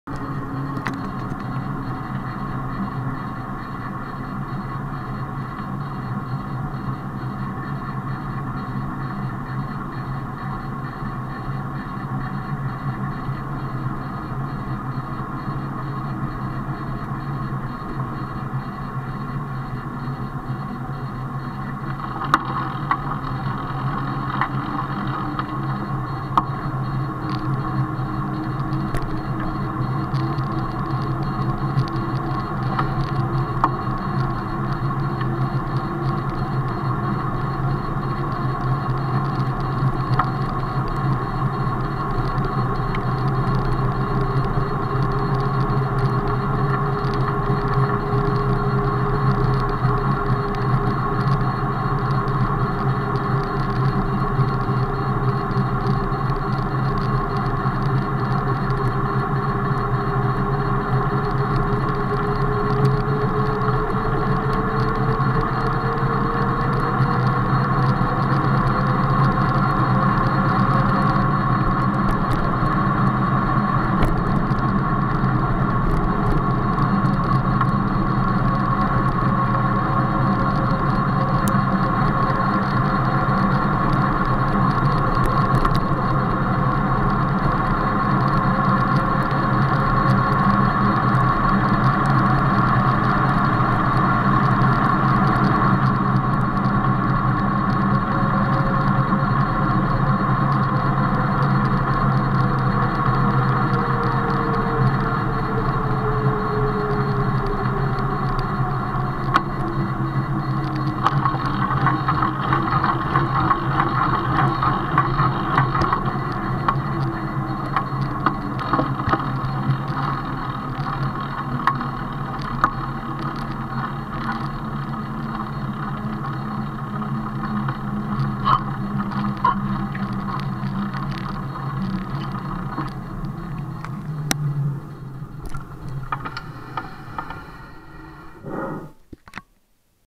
2nd run on the brutus
Riding to my friends Evans house